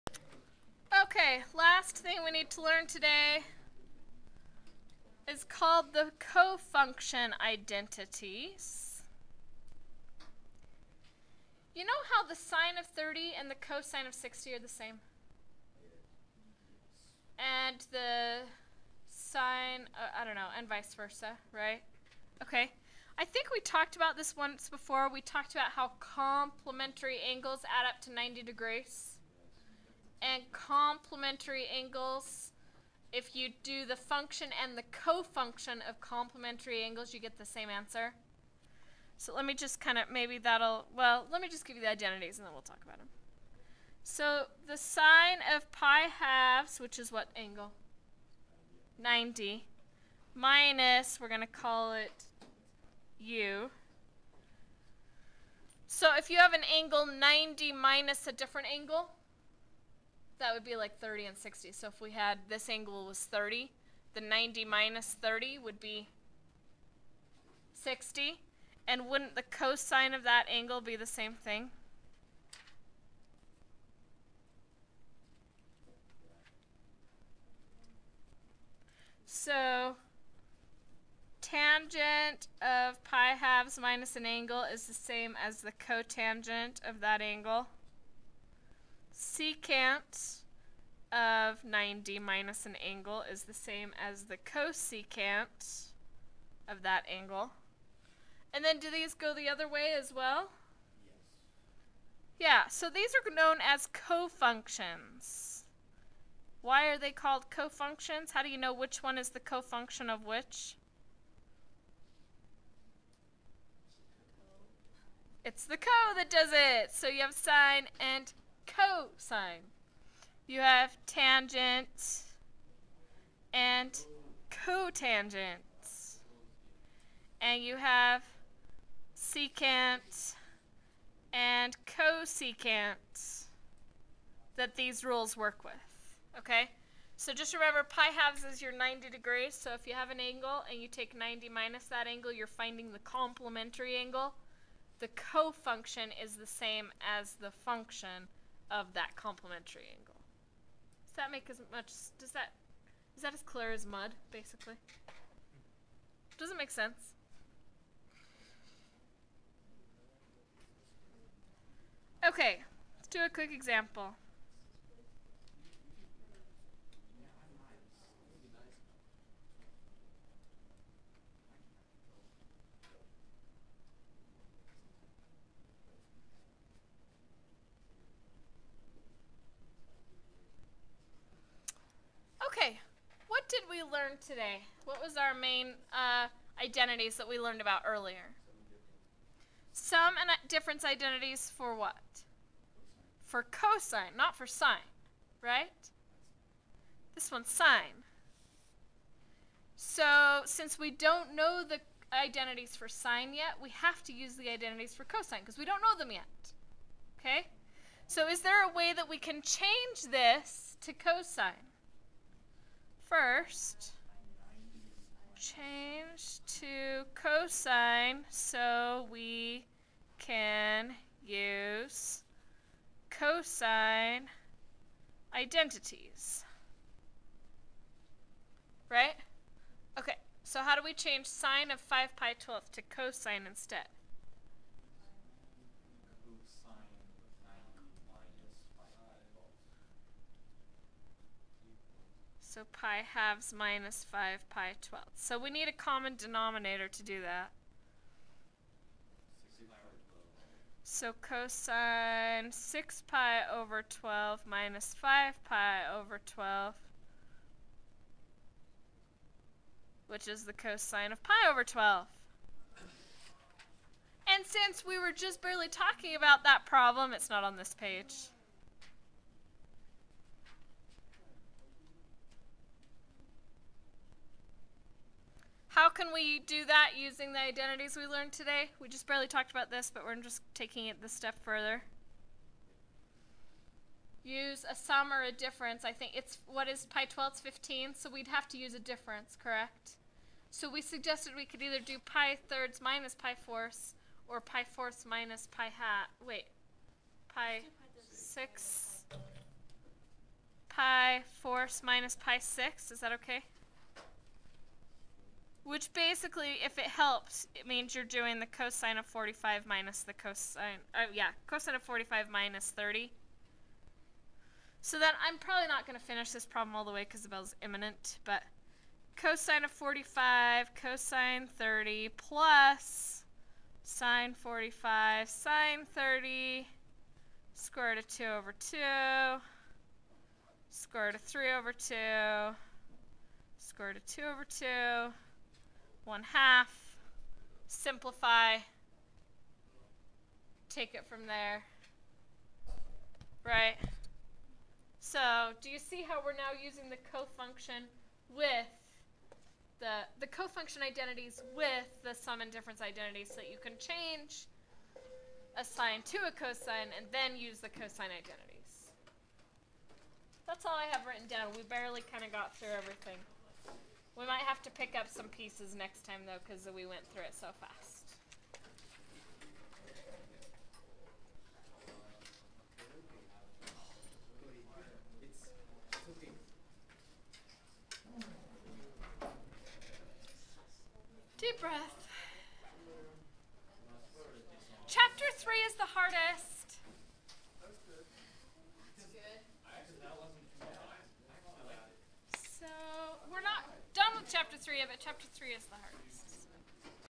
Trig Identities: Solving sin sum/difference problems by using cofunction identities.
Trig chapter 3 section 3, video part 3